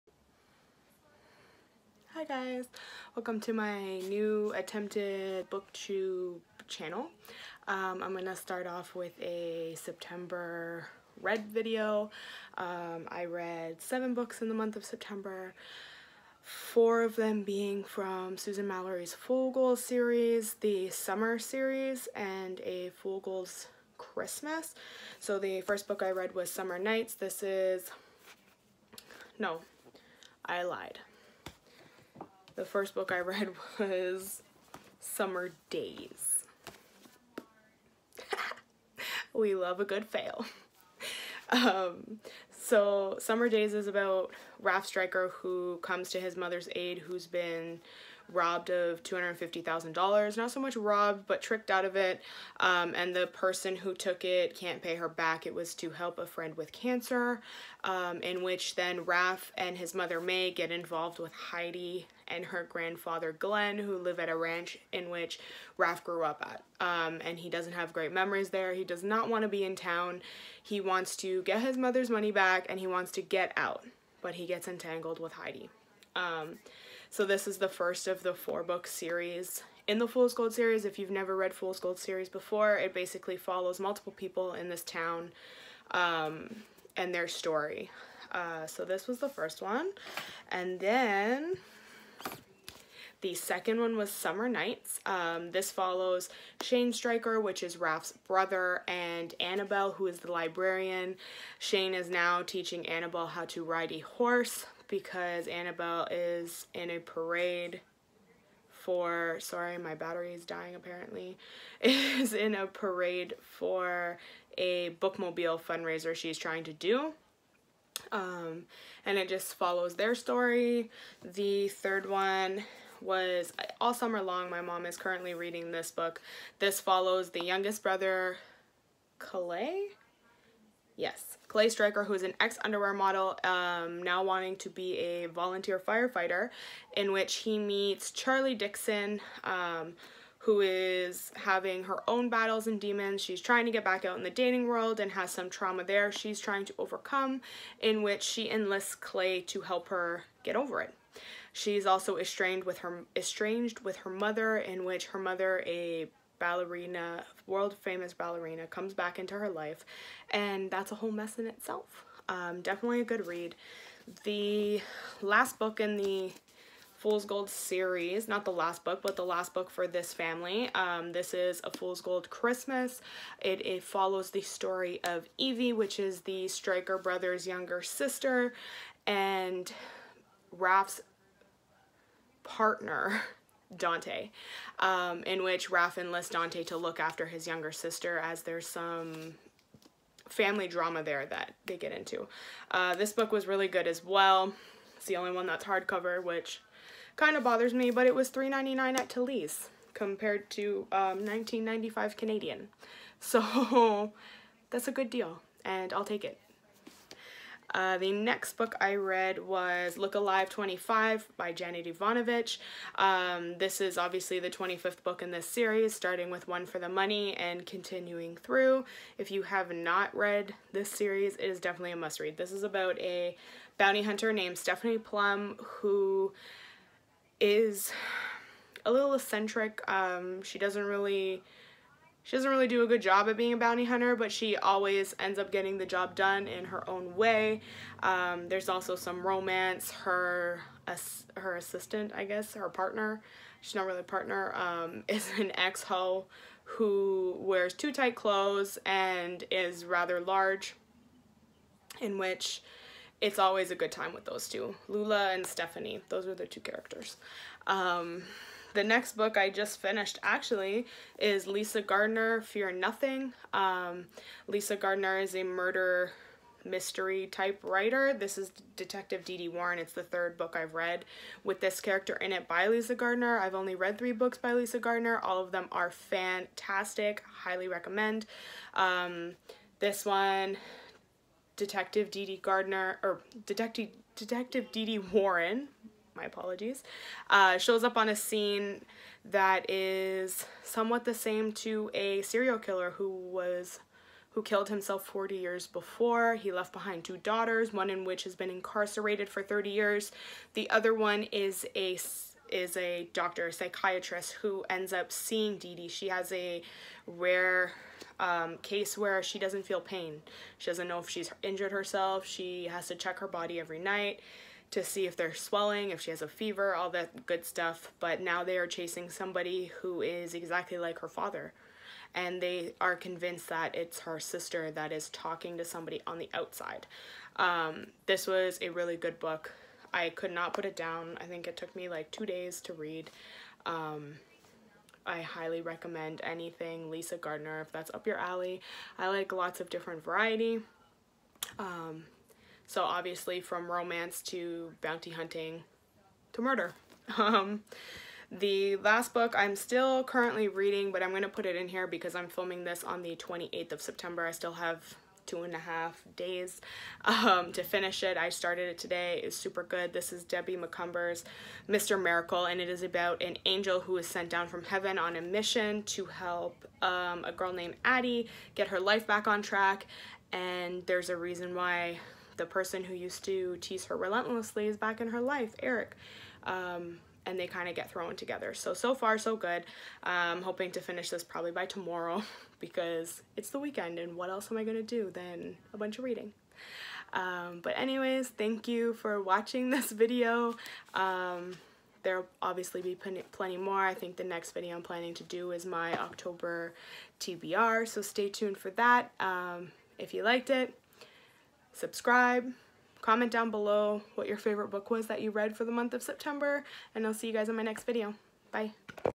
September Wrap Up 2019
Hey y’all

Books talked about in this video:

Mr. Miracle - Debbie Macomber

Happy reading (: